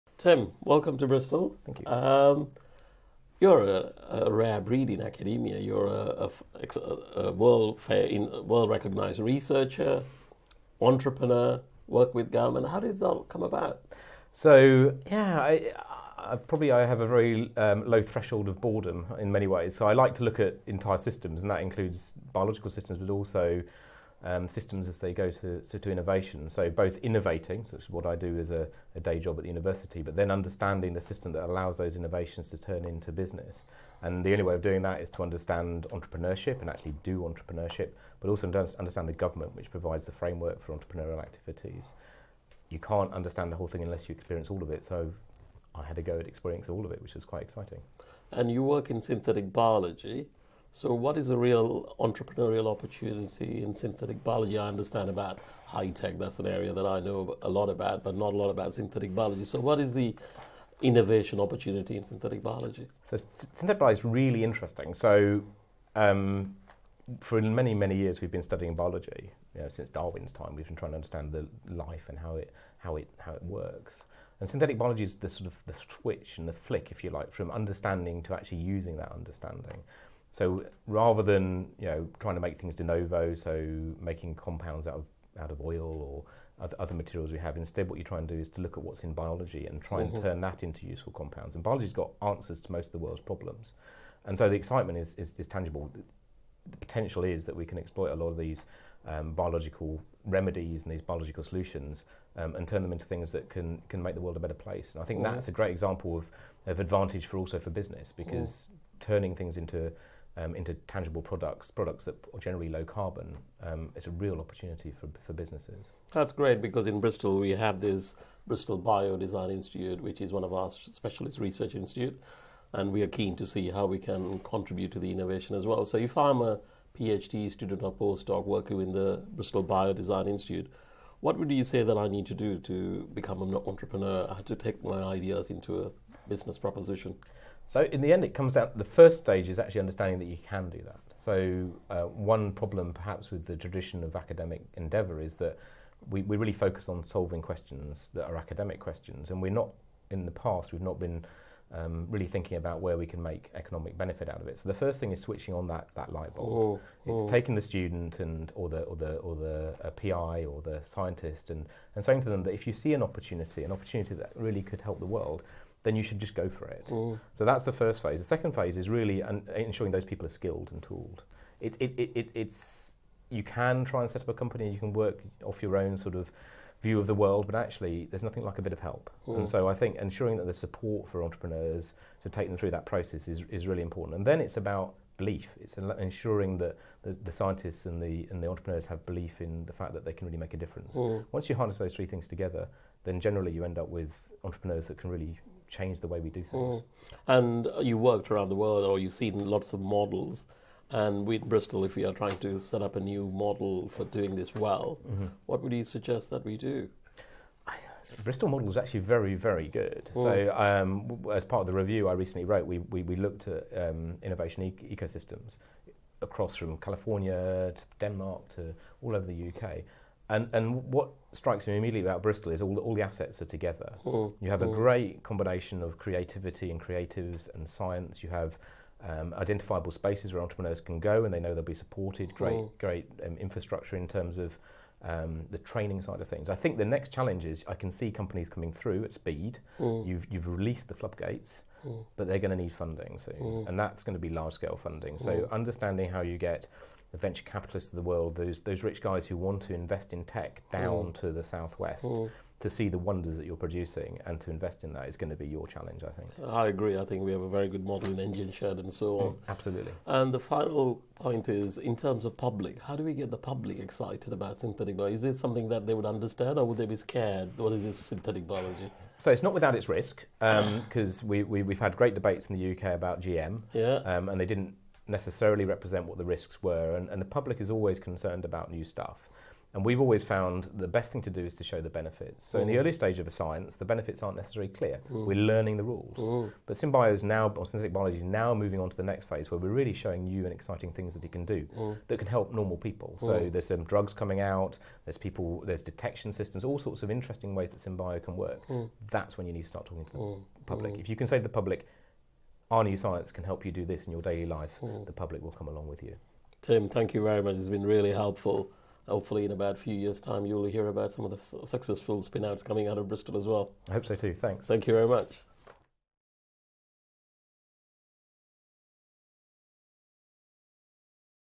Pro Vice-Chancellor Nishan Canagarajah talks to Tim Dafforn on Synthetic Biology
Professor Tim Dafforn visited the University of Bristol on the 17 November at the Life Science Building to give a seminar on Government policy vs real-world experience.

Professor Dafforn has held a number of high profile government positions including most recently, Chief Entrepreneurial Adviser at the Department for Business, Energy and Industrial Strategy (BEIS), Chief Scientific Adviser at the Department for Business, Innovation and Skills (BIS).

Alongside his Governmental duties, he runs a research group at the University of Birmingham where he is Director of Knowledge Transfer for the college of Life and Environmental Sciences. He is an expert in biophysical spectroscopy with a keen interest in synthetic biology and has developed a number of projects from inception through to commercial realisation.